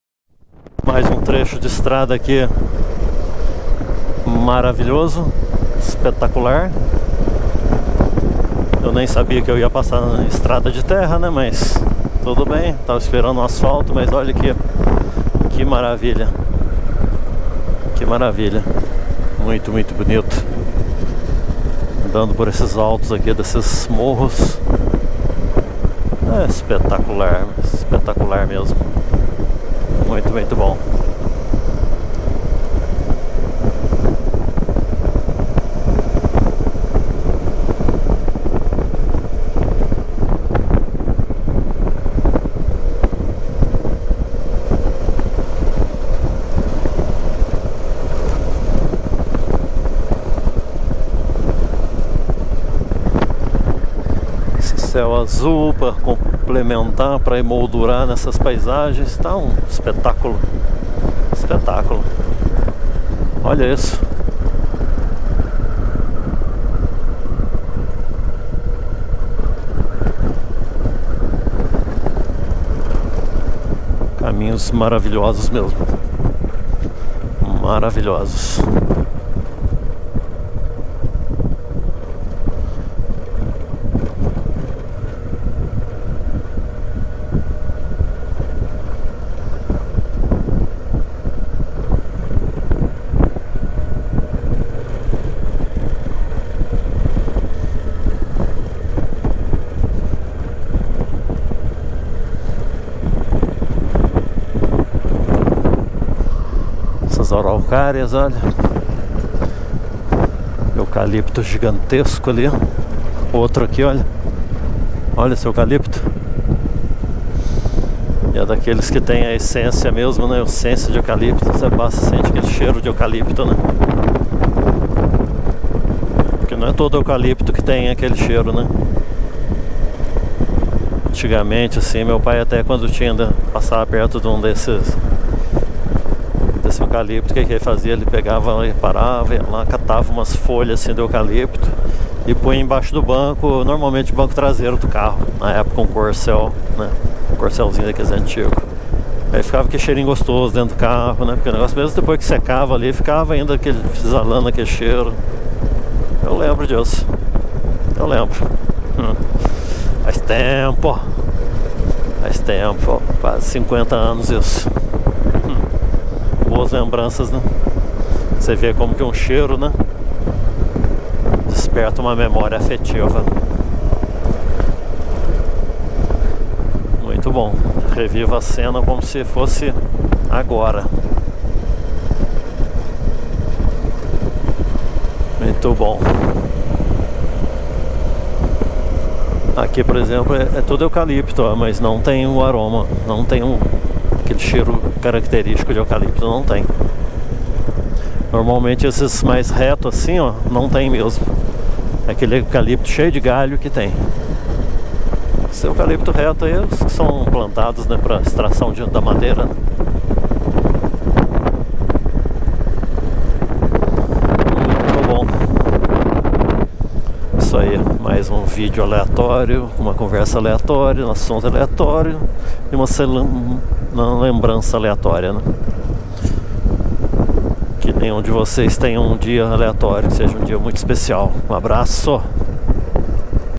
EXTREMO SUL DE MINAS GERAIS E MEMÓRIAS AFETIVAS.
Belíssimo trecho de estrada rural no extremo sul do Estado de Minas Gerais e conversando sobre memórias afetivas despertadas pelo perfume de eucaliptos.